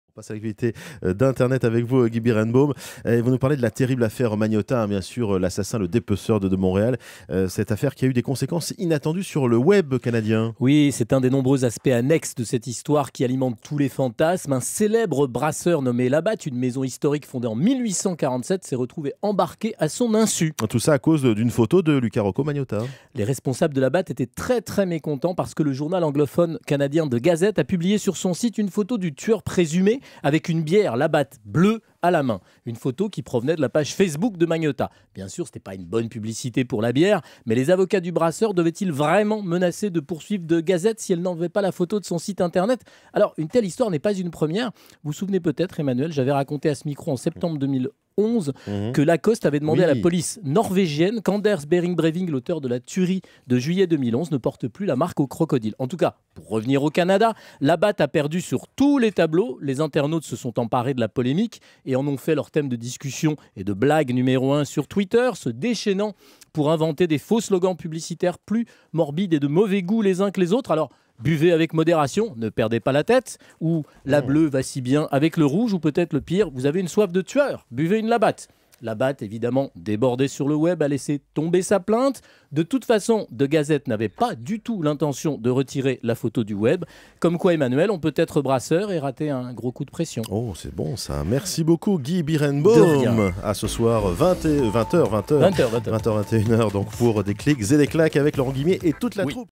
Trop de pression
Un brasseur nommé Labatt s'est retrouvé embarqué à son insu dans l'affaire Luka Rocco Magnotta à cause d'une photo.